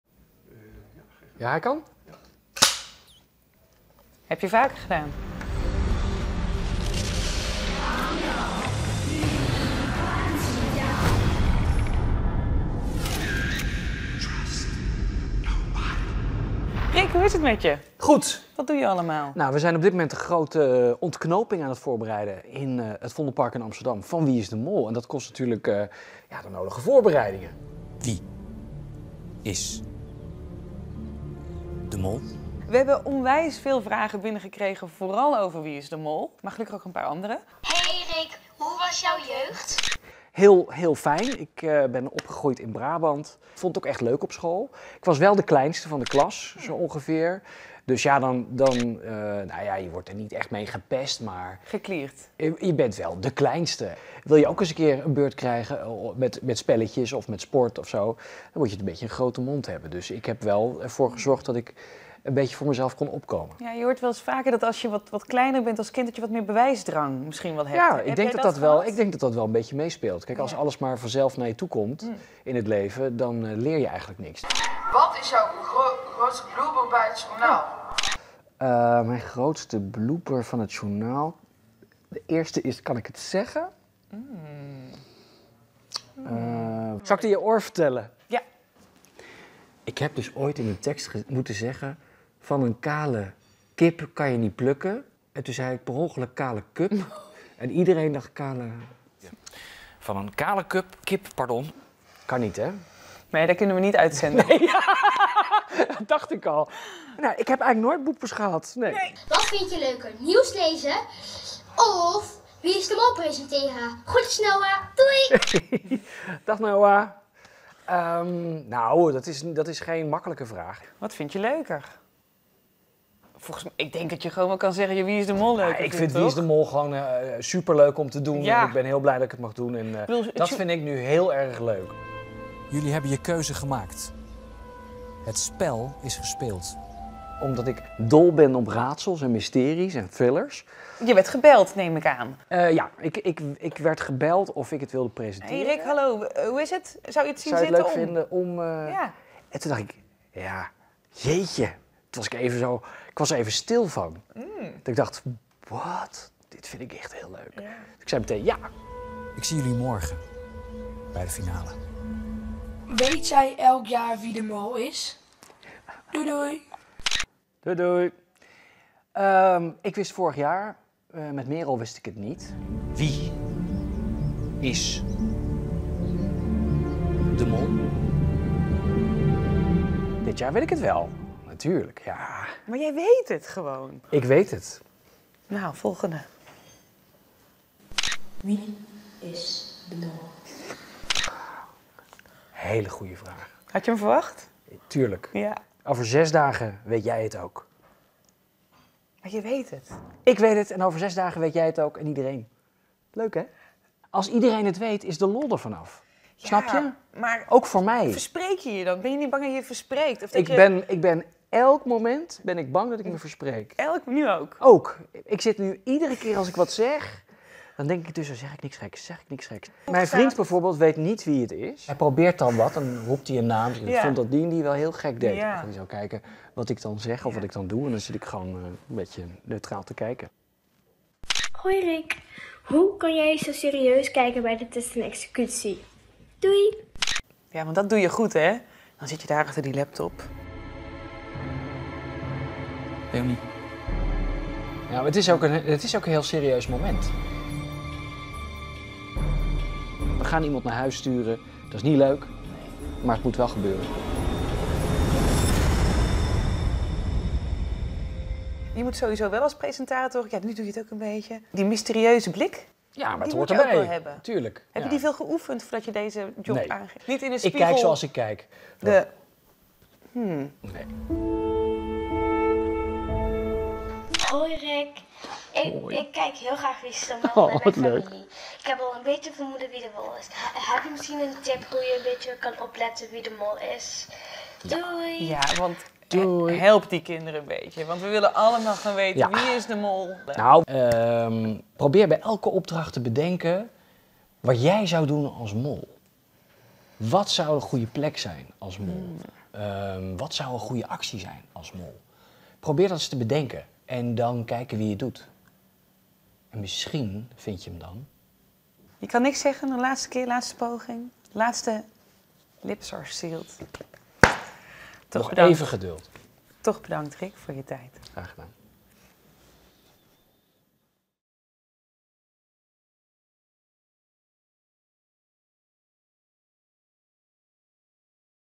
Rik van de Westelaken over de Mol en serieus kijken
2,5 miljoen mensen keken zaterdag naar Rik van de Westelaken in Wie is de Mol? Vroeger werkte hij bij het NOS Journaal en in die tijd deed hij zelf mee. Milou ging met jullie vragen bij hem langs. En natuurlijk ook met die ene vraag...